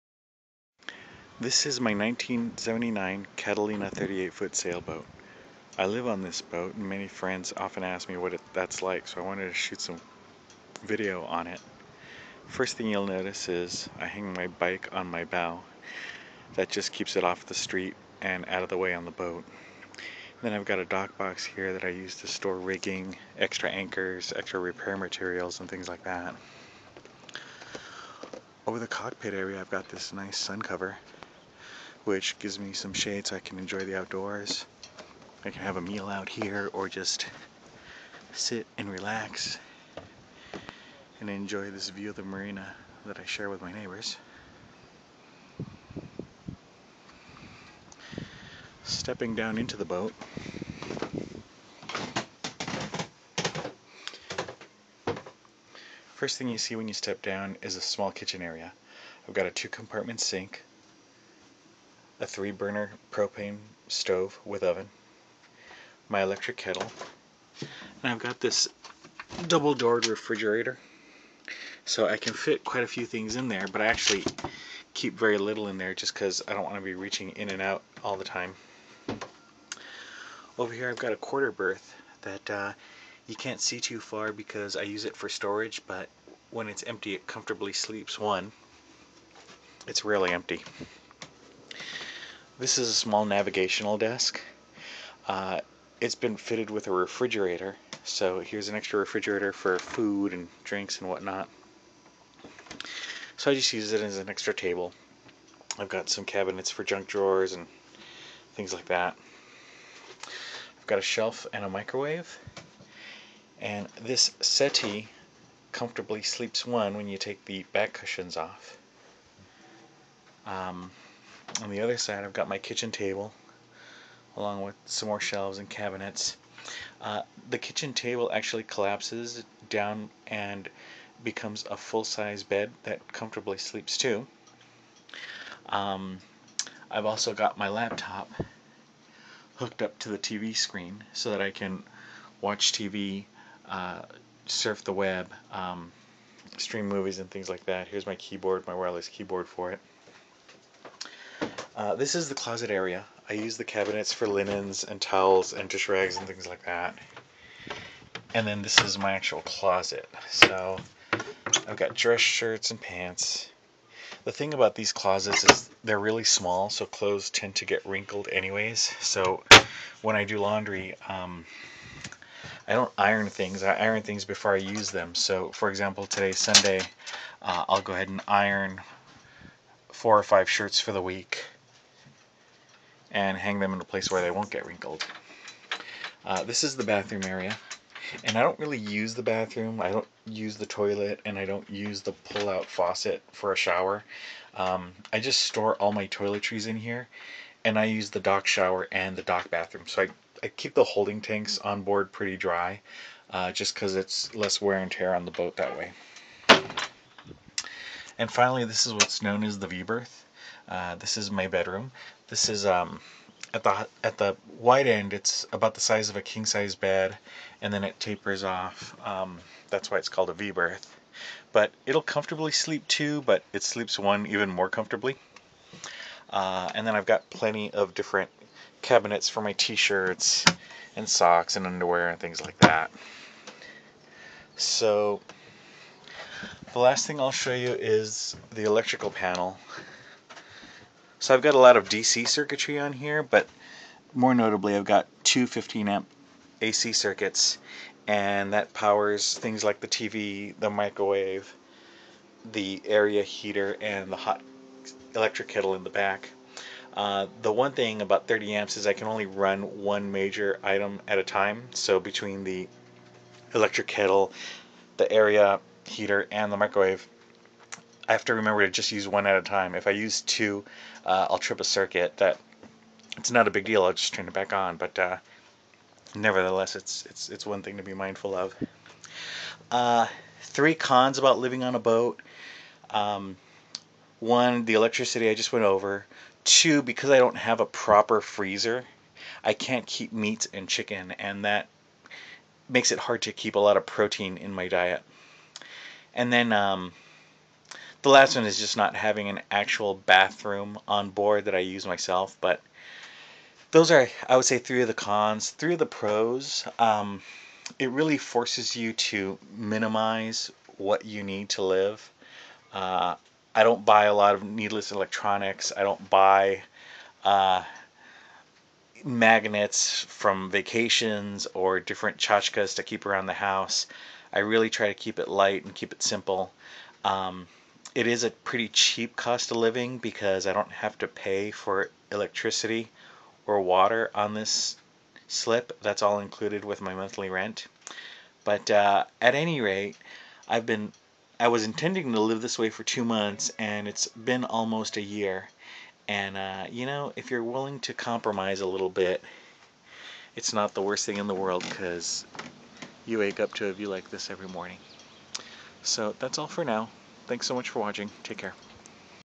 Living on a 1979 Catalina 38
Folks are always asking me what it's like to live on a boat, so here's a little video about it.

Found this video useful or entertaining? Want to thank me but don't know how?